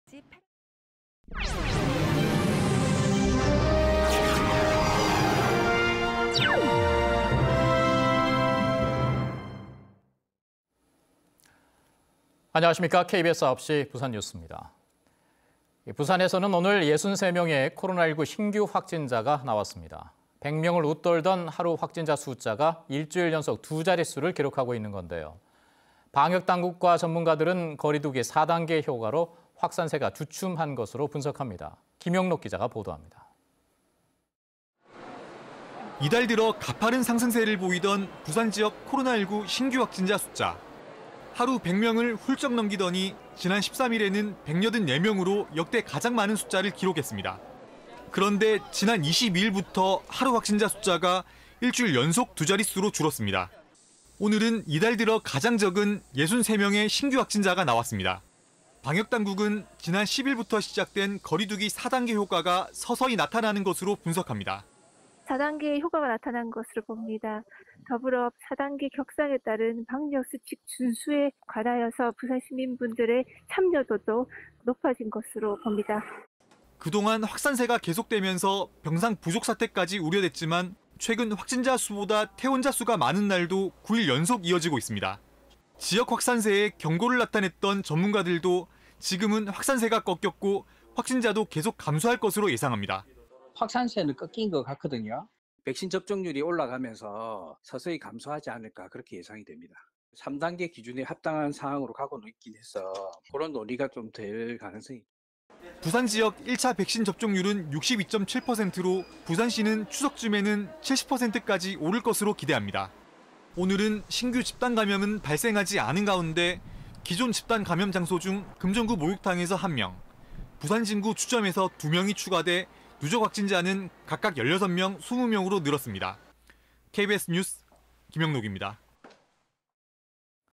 부산 일주일째 두 자릿수 확진…“4단계 효과” / KBS 2021.08.28.
[앵커]

 부산에서는 오늘 63명의 코로나19 신규 확진자가 나왔습니다.

 백 명을 웃돌던 하루 확진자 숫자가 일주일 연속 두 자릿수를 기록하고 있는 건데요.

 방역당국과 전문가들은  거리두기 4단계 효과로  확산세가 주춤한 것으로 분석합니다.

 김영록 기자가 보도합니다.

 [리포트]

 이달 들어  가파른 상승세를 보이던  부산지역 코로나19 신규확진자 숫자.

 하루 백 명을 훌쩍 넘기더니  지난 13일에는 184명으로 역대 가장 많은 숫자를 기록했습니다.

 그런데 지난 22일부터 하루 확진자 숫자가 일주일 연속 두 자릿수로 줄었습니다.

 오늘은 이달 들어 가장 적은 63명의 신규 확진자가 나왔습니다.

 방역당국은 지난 10일부터 시작된  거리두기 4단계 효과가  서서히 나타나는 것으로 분석합니다.

 [이정민/부산시 감염병대응팀장 : "(확산세 감소는) 4단계 효과가 나타난 것으로 봅니다. 더불어 4단계 격상에 따른 방역수칙 준수에 관하여 부산 시민분들의 참여도도 높아진 것으로 봅니다."]

 그동안 확산세가 계속되면서 병상 부족 사태까지 우려됐지만  최근 확진자 수보다 퇴원자 수가 많은 날도 9일 연속 이어지고 있습니다.

 지역 확산세에 경고를 나타냈던  전문가들도 지금은 확산세가 꺾였고, 확진자도 계속 감소할 것으로 예상합니다.

 [정동식/동아대병원 감염내과 교수 : "확산세는 꺾인 것 같거든요. 백신 접종률이 올라가면서 서서히 (확진자 숫자가) 감소하지 않을까 그렇게 예상이 됩니다. 3단계 기준에 합당한 상황으로 가고는 있긴 해서 그런 논의가 좀 될 가능성이…."]

 부산지역 1차 백신 접종률은 62.7%로,  부산시는 추석쯤에는  70%까지 오를 것으로 기대합니다.

 오늘은 신규 집단 감염은 발생하지 않은 가운데, 기존 집단 감염 장소 중  금정구 목욕탕에서 1명, 부산진구 주점에서 2명이 추가돼 누적 확진자는 각각 16명,20명으로 늘었습니다.

 KBS 뉴스 김영록입니다.

영상편집:백혜리